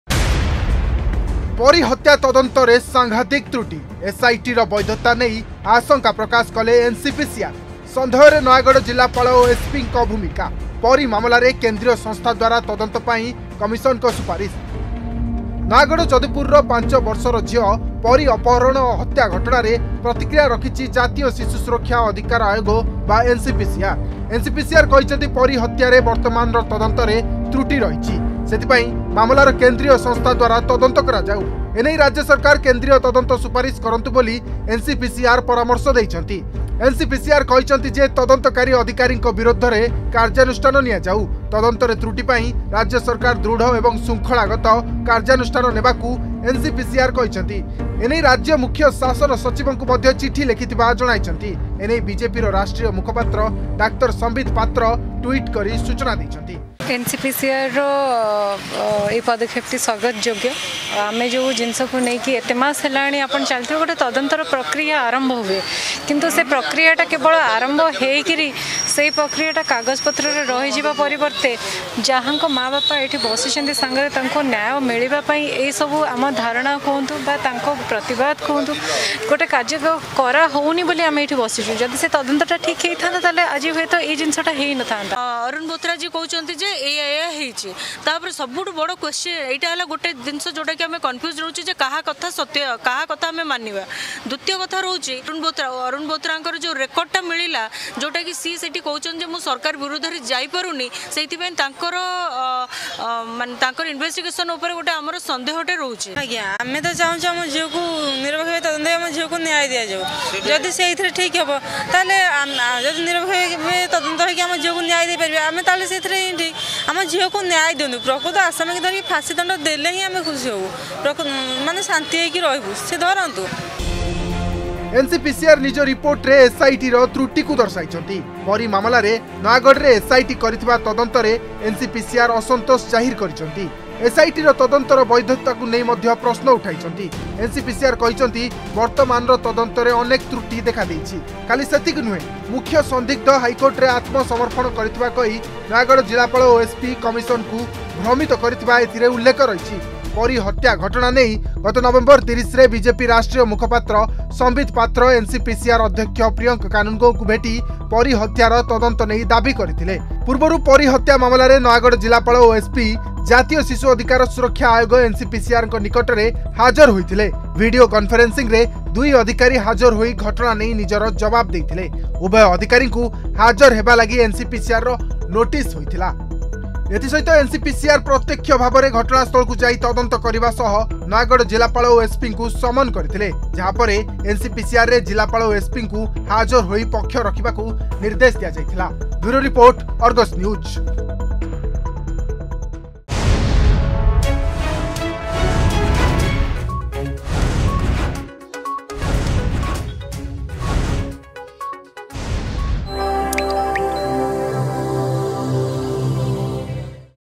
ପରୀ ହତ୍ୟା ତଦନ୍ତରେ ରହିଛି ସଂଘାତିକ ତ୍ରୁଟି ..
ପରୀ ହତ୍ୟା ତଦନ୍ତରେ ରହିଛି ସଂଘାତିକ ତ୍ରୁଟି । ଏସଆଇଟିର ନିରପେକ୍ଷ ତଦନ୍ତକୁ ନେଇ ଆଶଙ୍କା ପ୍ରକାଶ କରିବା ସହ ପରୀ ହତ୍ୟା ମାମଲାର କେନ୍ଦ୍ରୀୟ ଏଜେନ୍‌ସୀ ଦ୍ବାରା ତଦନ୍ତ କରାଇବା ପାଇଁ ସୁପାରିଶ କରିଛନ୍ତି ଜାତୀୟ ଶିଶୁ ସୁରକ୍ଷା ଓ ଅଧିକାର ଆୟୋଗ-ଏନ୍‌ପିସିଆର .